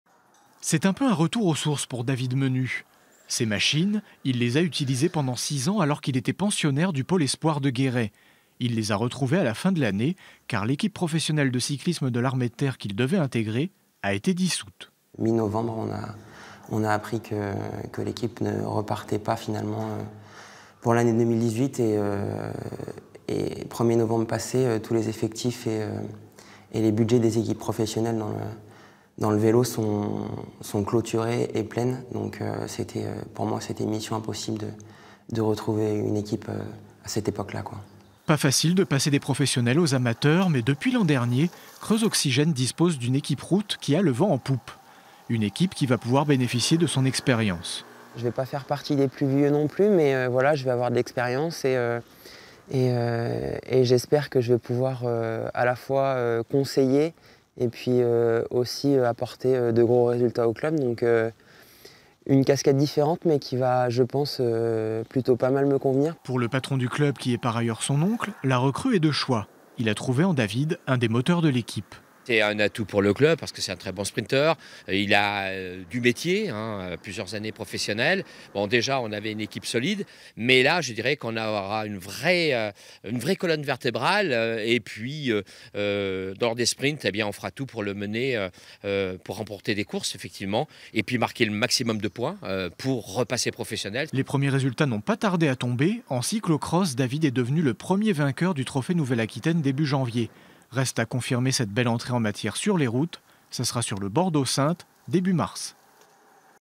Le cycliste creusois David Menut roule en amateur
Avec la dissolution fin 2017 de l'équipe professionnelle Armée de Terre, 20 coureurs restaient sur le carreau. David Menut était parmi eux. Le creusois, chez les pros depuis 3 ans s'est retrouvé démuni et a été contraint de signer chez les amateurs dans l'équipe DN1 route de son oncle, Creuse oxygène. Intervenants : David Menut cycliste Creuse oxygène, Alain Menut Président de Creuse Oxygène. Equipe : Nicolas Chigot, Frédérique Bordes, Alain Lafeuille.